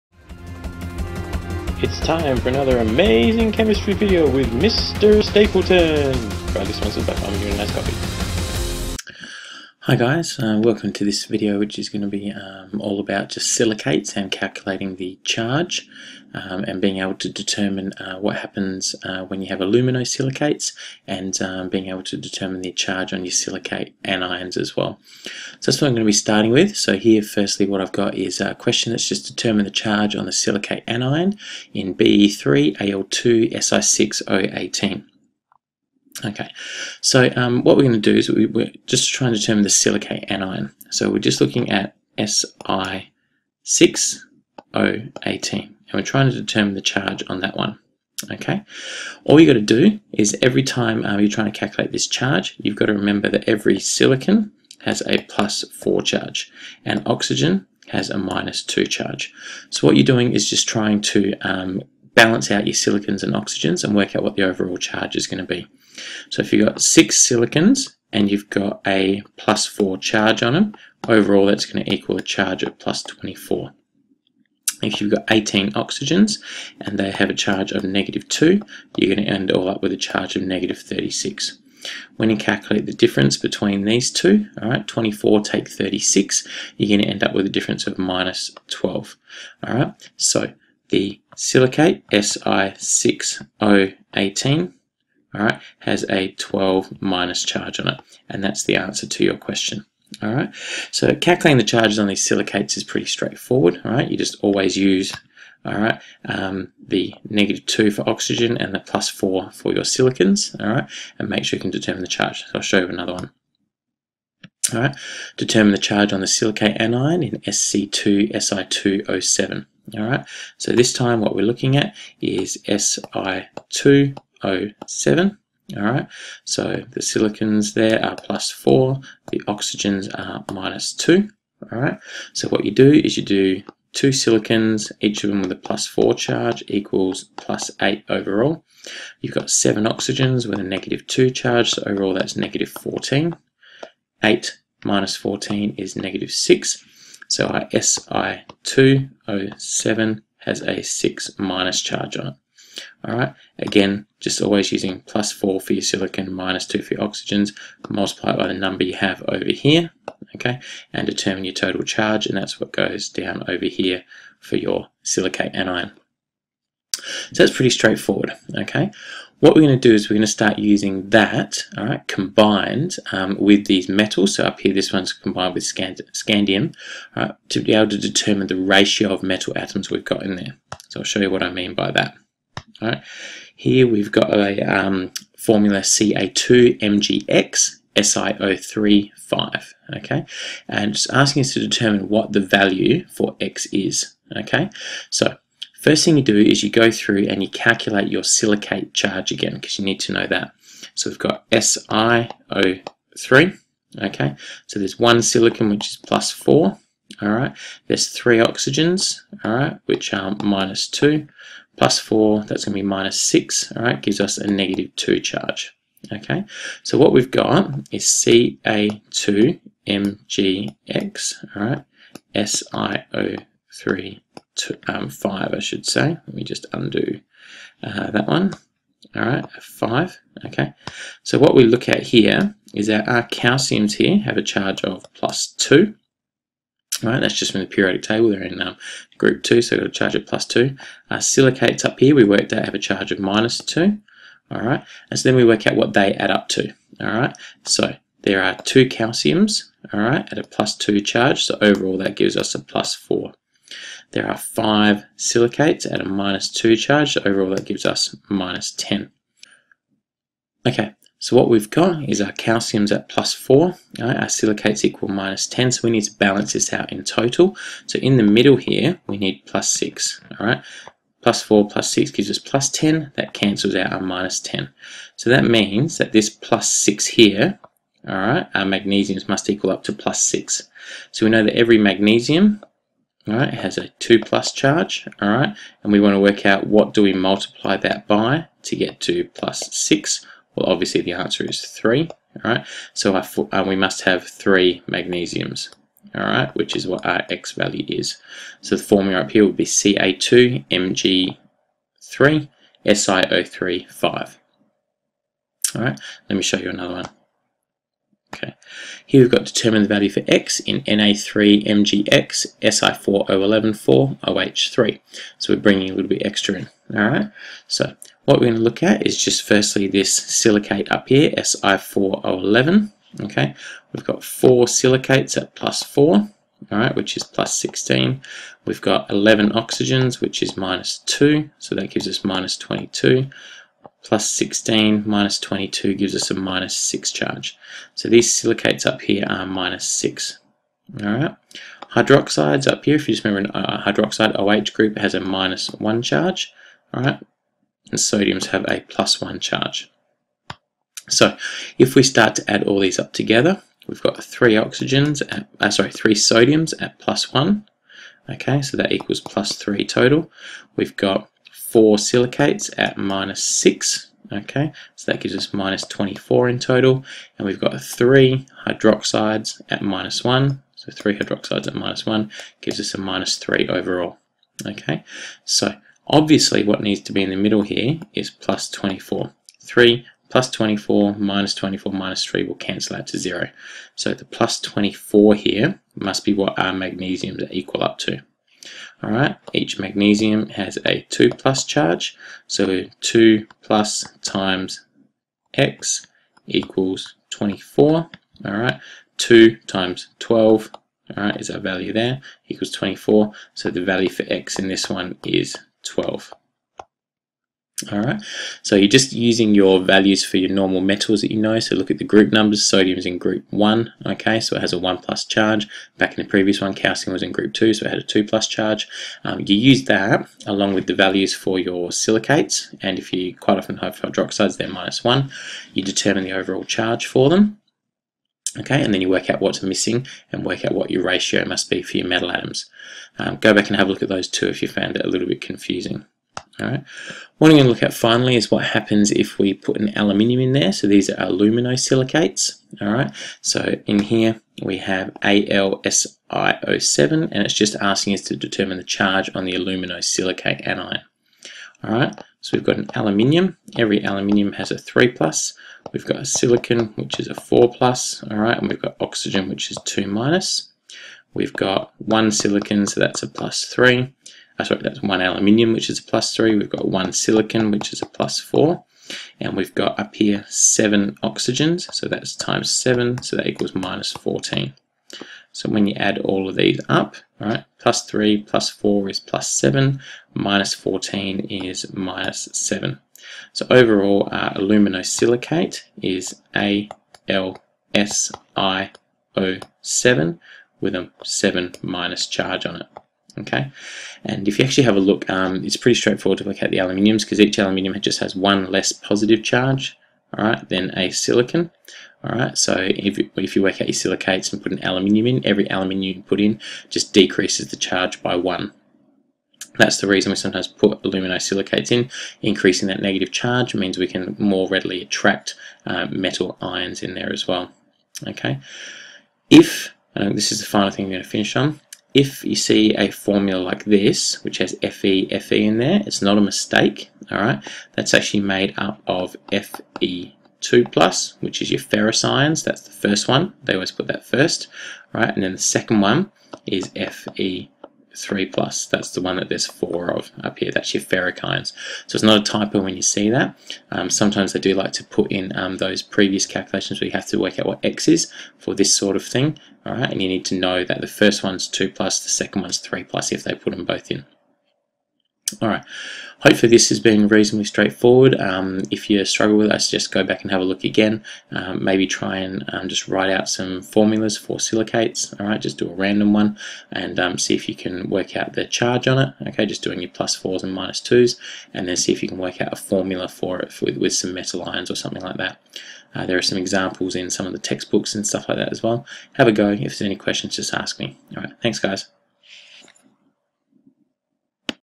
Calculating Silicate Charges
How to calculate charges on silicate anions, determine ratio of metal atoms and aluminosilicates